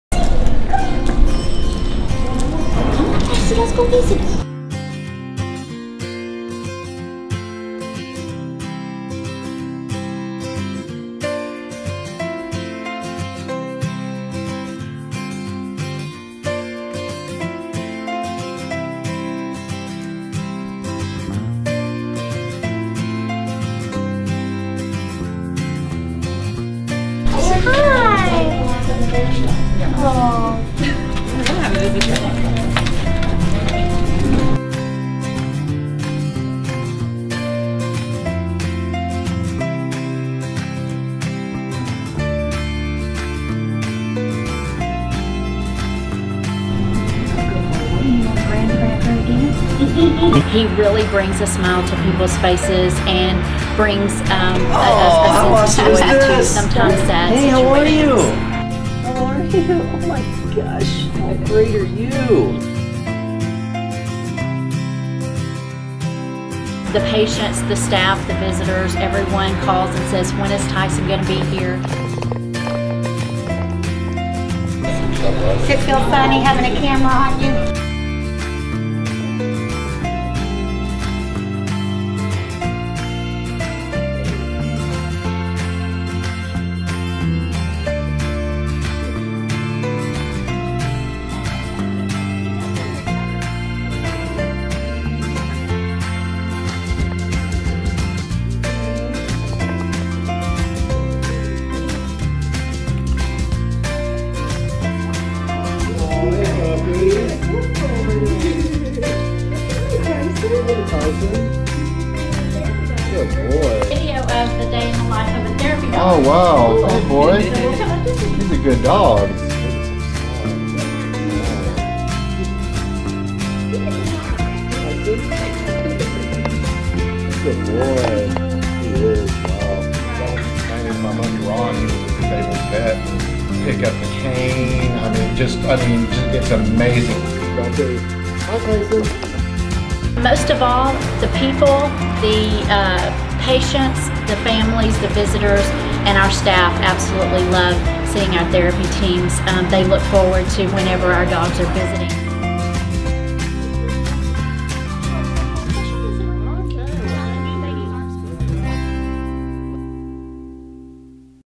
Day in the Life of a Therapy Dog
Tyson's dogs-eye-view of a day in the life of a therapy dog at Medical City Denton